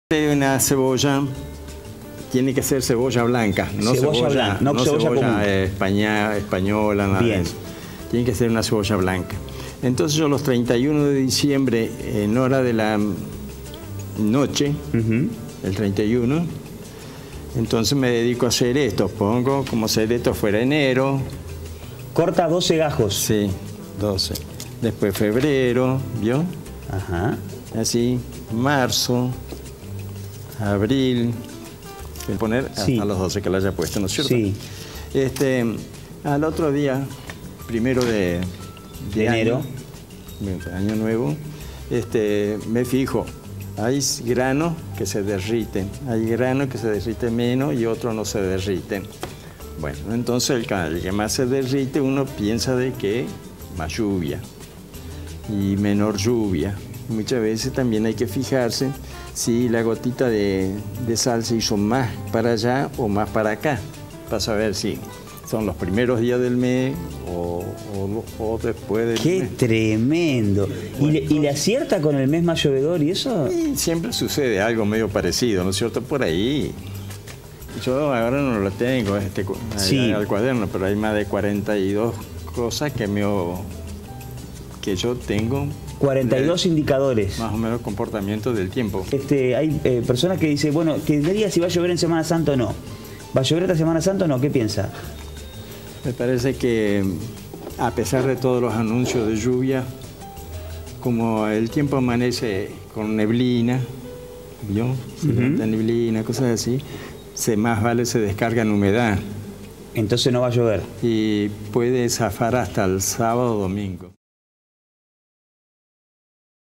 El señor de la lluvias tiene 92 años y desde hace 77 registra los milímetros caídos
Carlos Chiapino es un apasionado del tiempo y uno de los fundadores de la capilla del barrio de Kilómetro 5 y 1/2